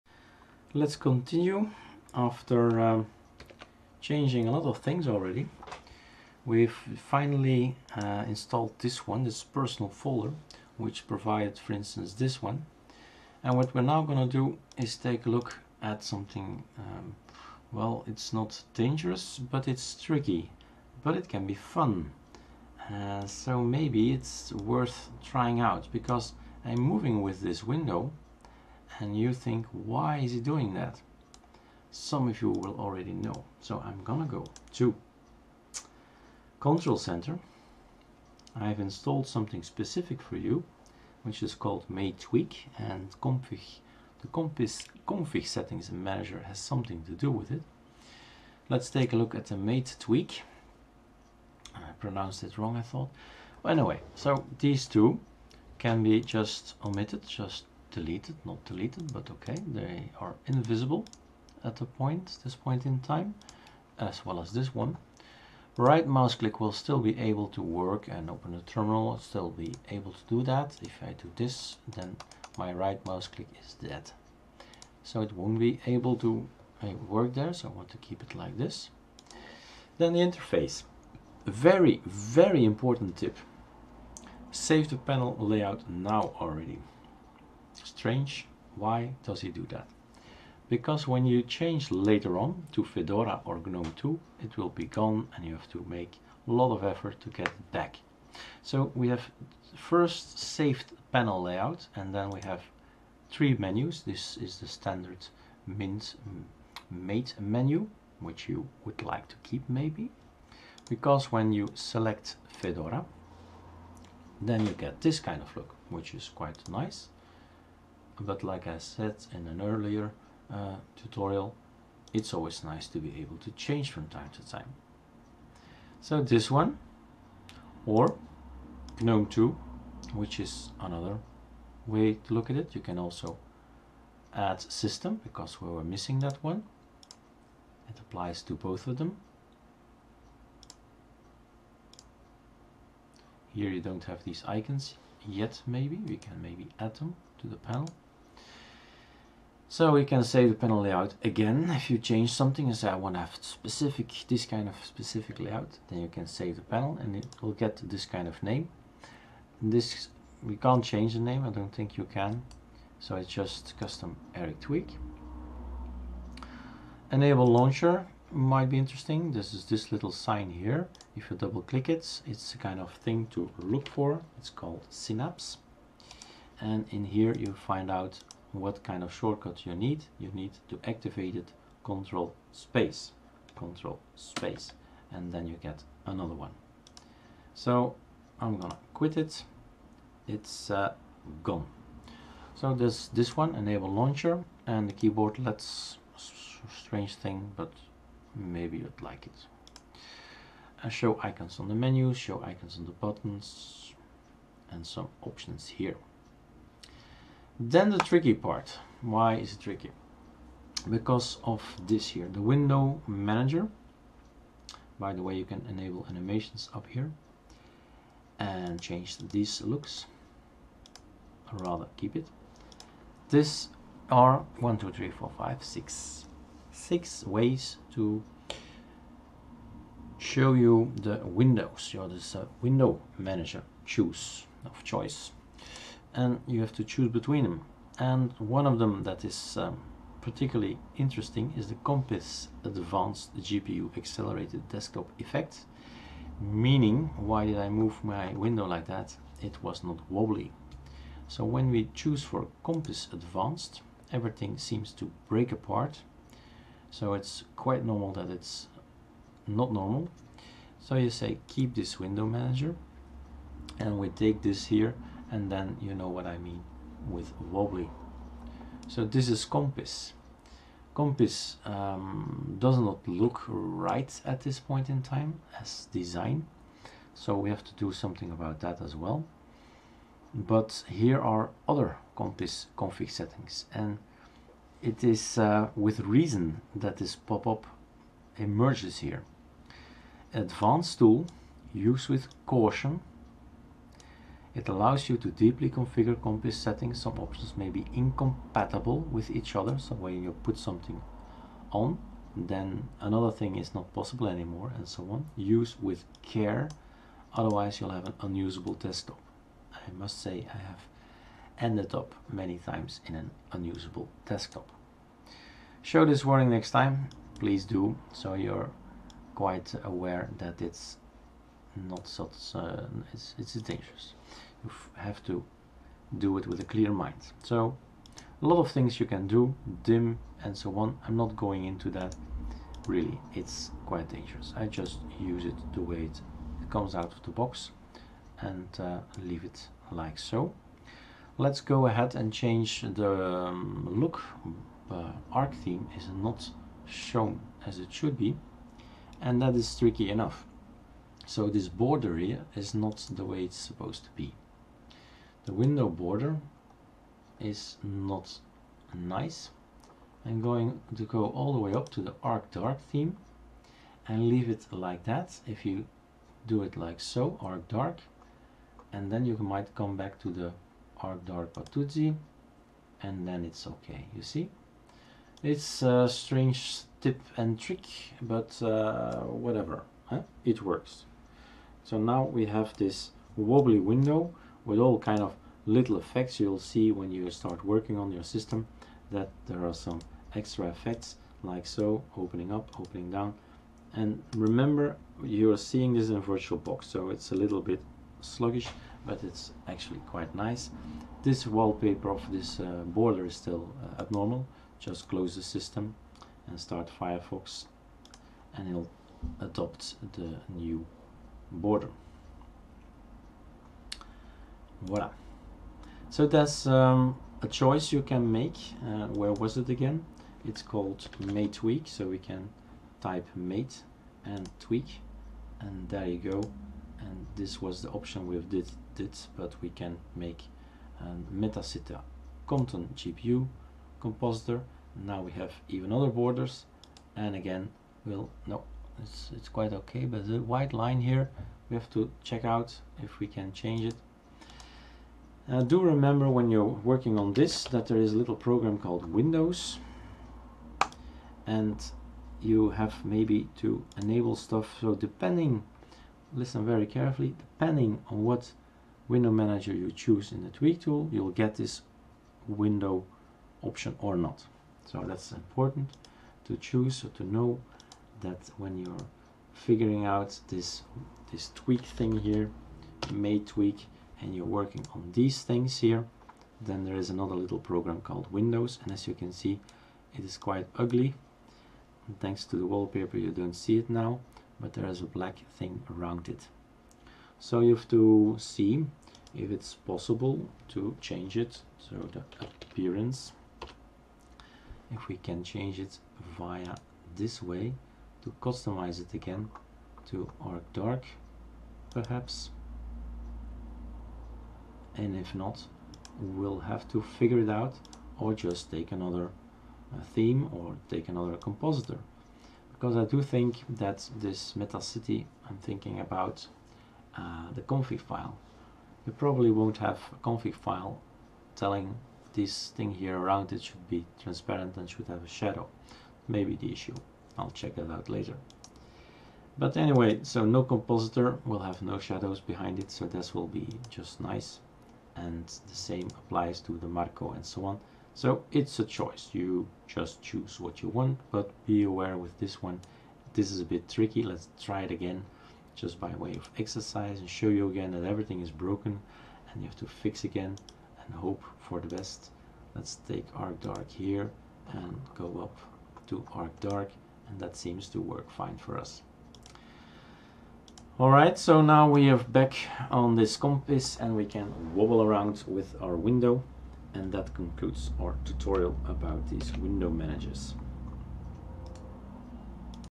How to install Linux Mint 18.1 Mate and redesign it - 6. choosing a window manager
Complete tutorial series to finetune your mate system.
This video is part of a tutorial A step by step guide how to install Linux Mint 18.1 Mate and how to tune it the way you want it.